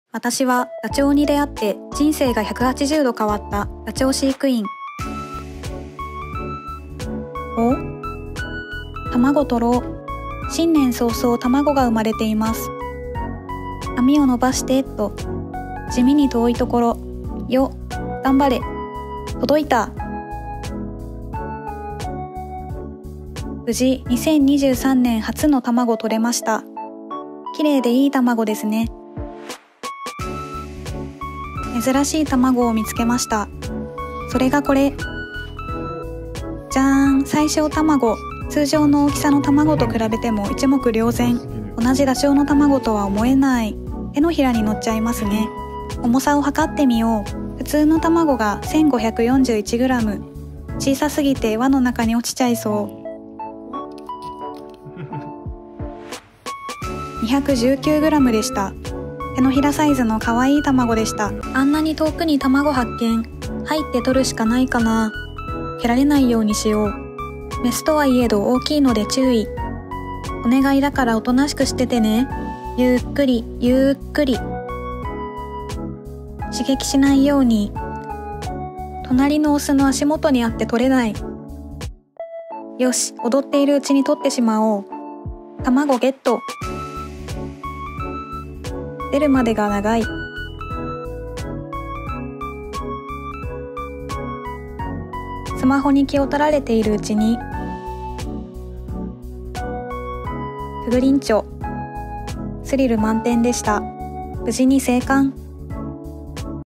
ダチョウの卵取り総集編PART.4【美里オーストリッチファーム】ダチョウ
美里オーストリッチファームでのダチョウの卵取り集PART.5です。